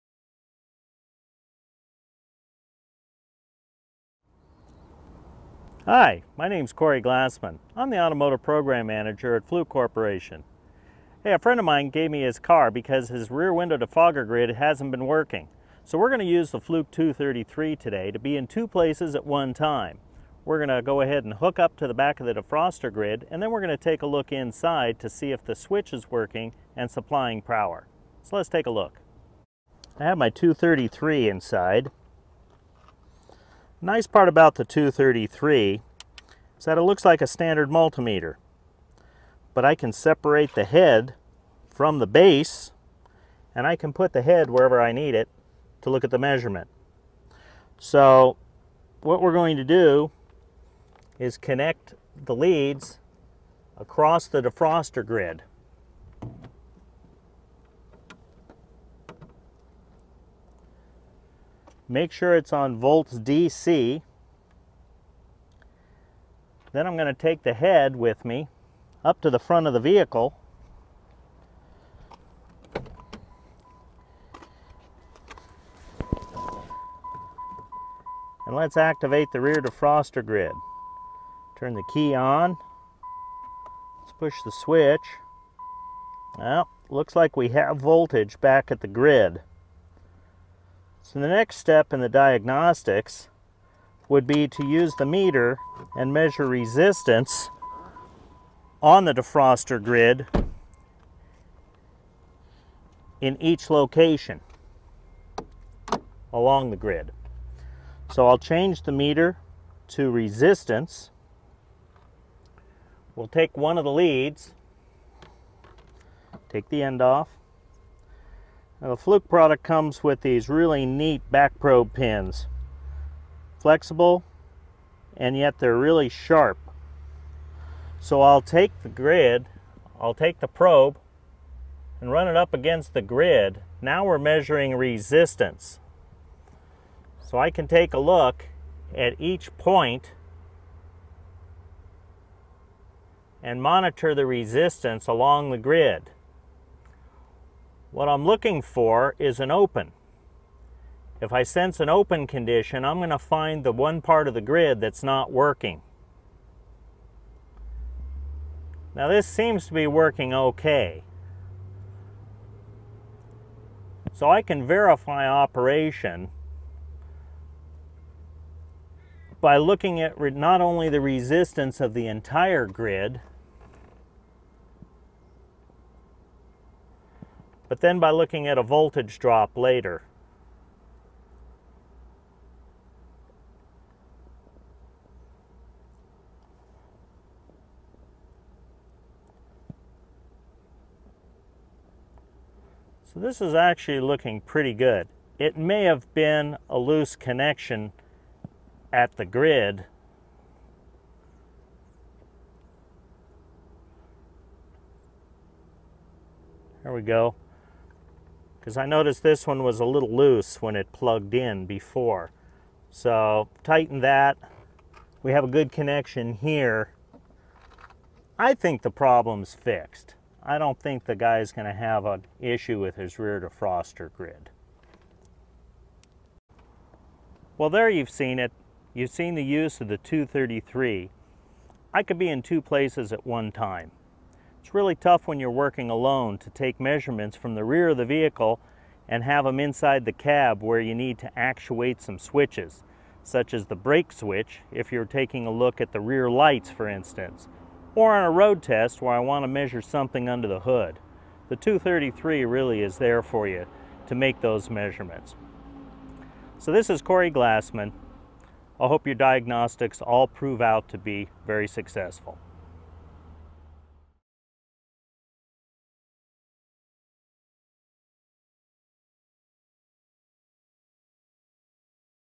FLUKE 233 Multimeter with remote Display. Application: Analysing/Testing The Rear Window Defroster
FLUKE 233 Remote Display Multimeter.

 An Automotive Specialist checks out the rear window defroster on a friend's car.  Watch Corey use his FLUKE 233 Wireless Multimeter to connect at the rear window, detach the readout and then sit up front and verify the voltage readings as he switches the defroster on/off.